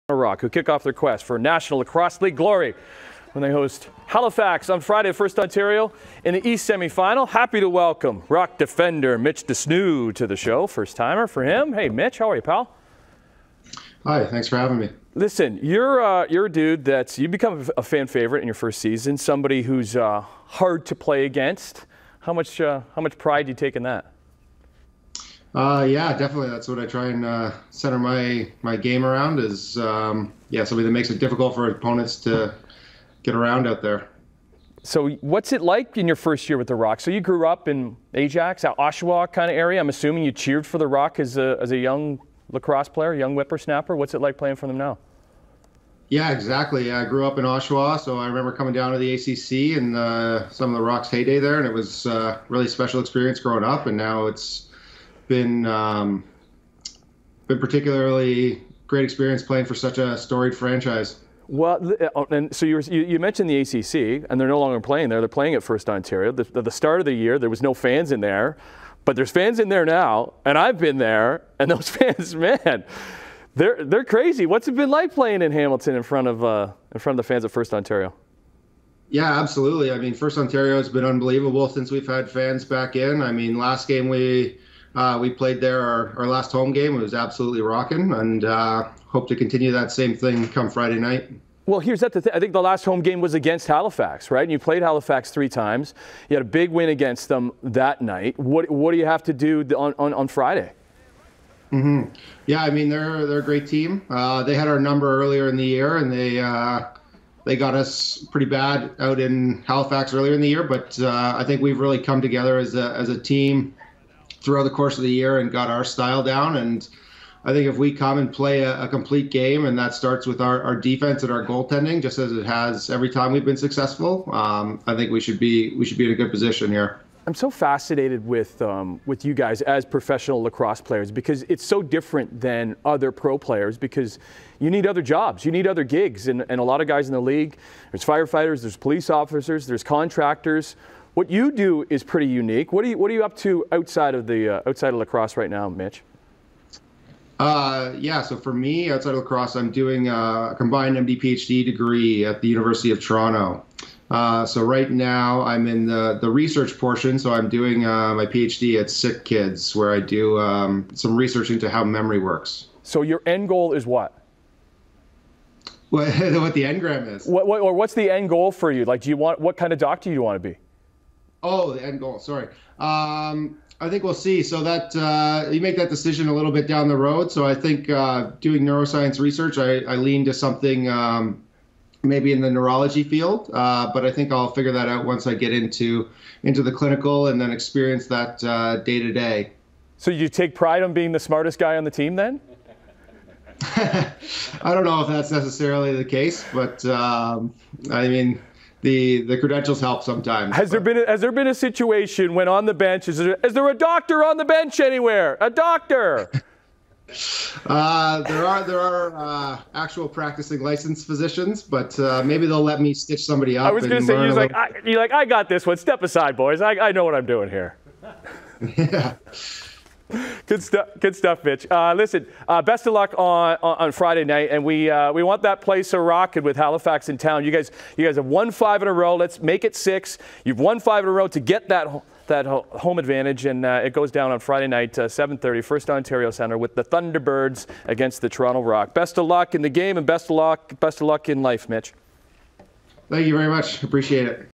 Toronto Rock host Halifax Thunderbirds in East Conference Semi-Final
There is no shortage of options for sports fans this time of the year with the Toronto Maple Leafs and Hamilton Bulldogs in the playoffs, but don't forget about the Toronto Rock.  They kick off their quest for National Lacrosse League glory when the host Halifax on Friday at FirstOntario Centre.  This morning we welcomed Rock defender Mitch de Snoo to Morning Live.